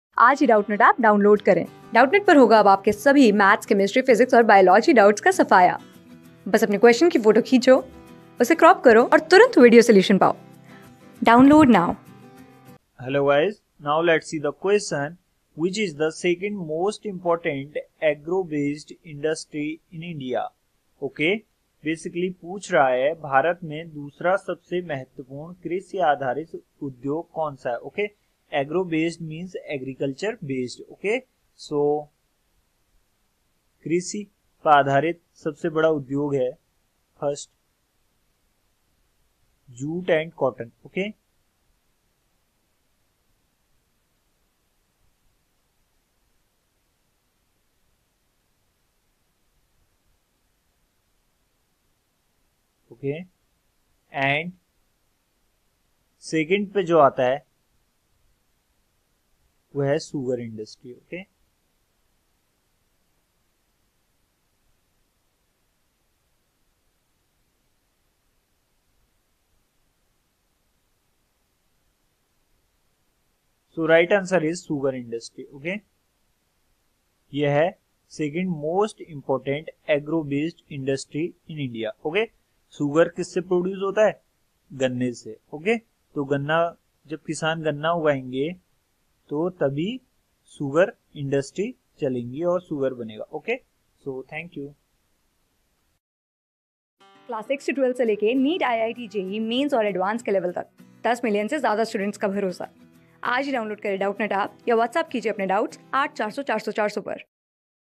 Which is the 2nd most important agro-based industry in India? | CLASS 12 | MANUFACTURING INDUSTR...
Which is the 2nd most important agro-based industry in India?
Class: 12
Subject: GEOGRAPHY
Chapter: MANUFACTURING INDUSTRIES
Board:CBSE

👉 Have Any Query? Ask Us.